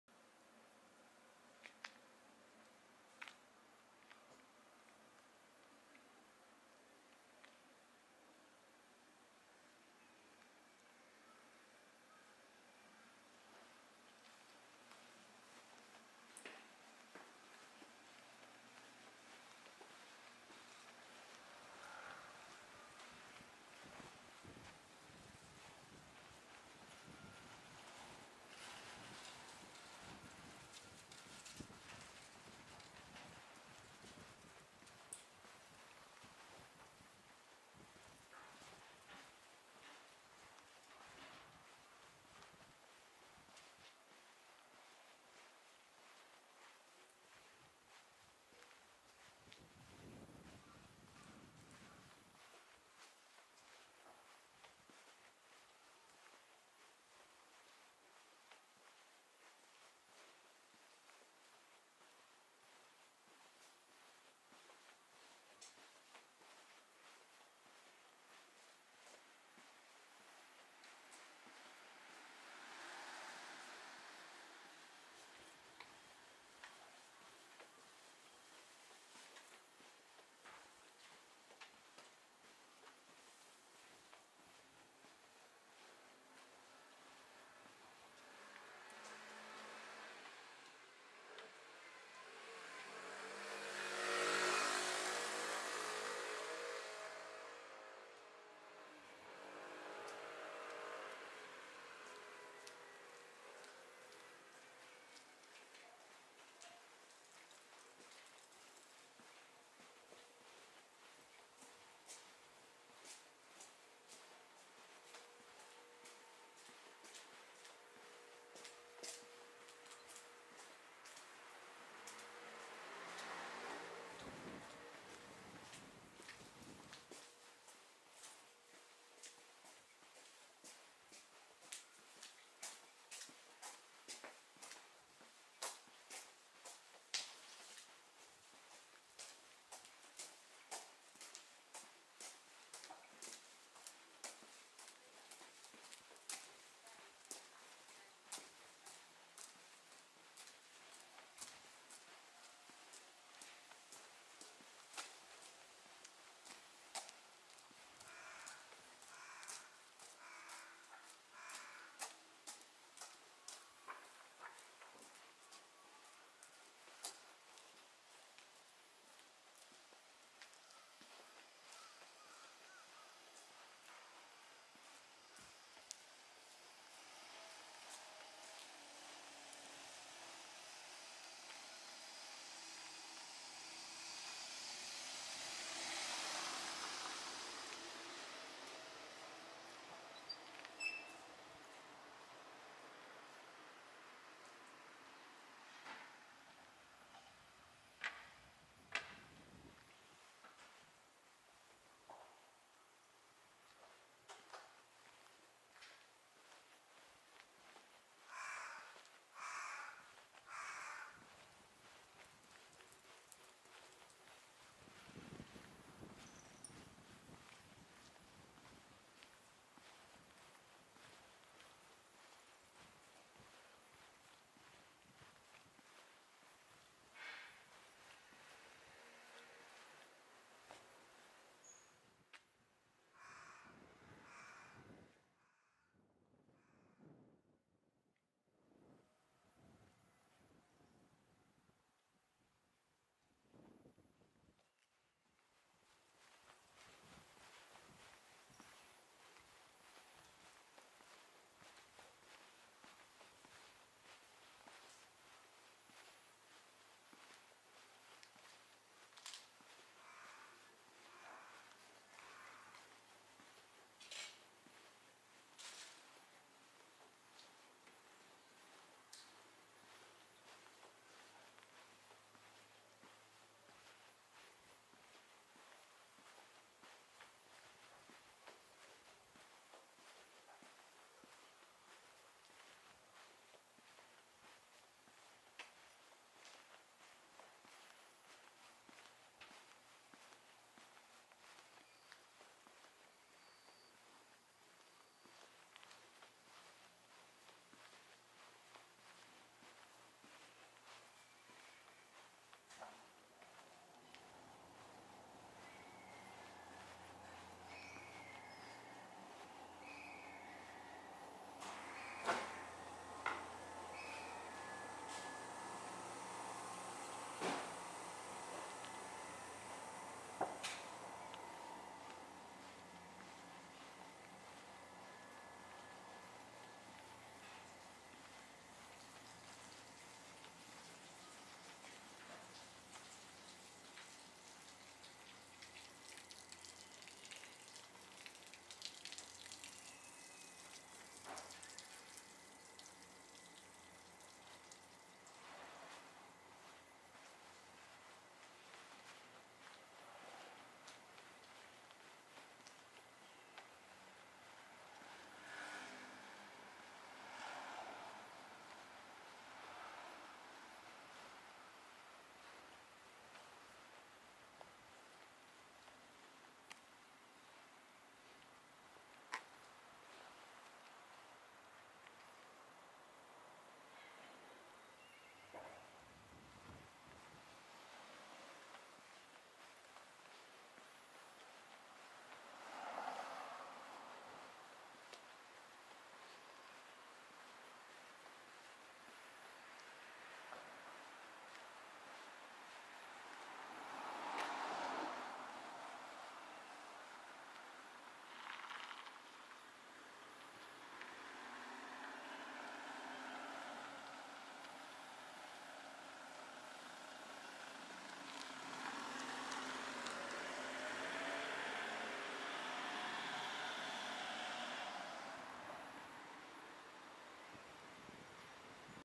尾鷲の街並み 16 三重県尾鷲市
尾鷲の街並み 16
三重県尾鷲市中井町,港町
撮影 2018年11月20日 火曜日 12時50分
Owase Mie Japan